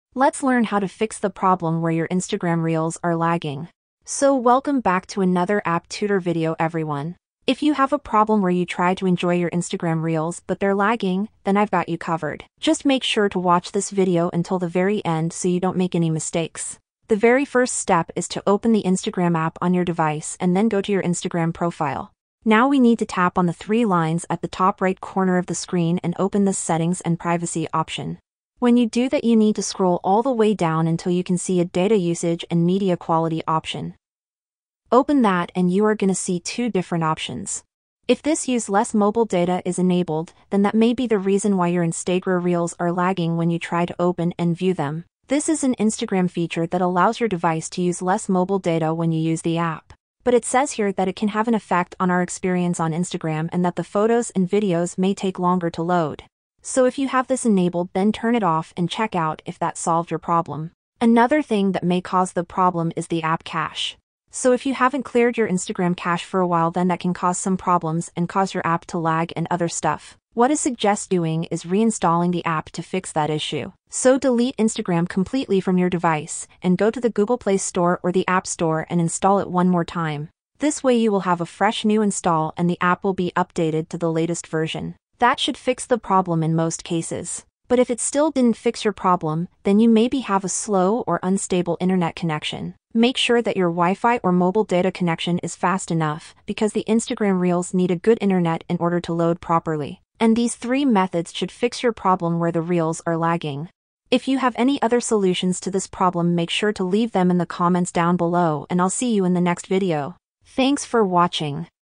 How to Fix Instagram Reels Lag
Are you tired of experiencing frustrating lag while trying to enjoy Instagram Reels? In this video, we have compiled a range of effective methods to help you fix Instagram Reels lag and enjoy a smooth viewing experience once again.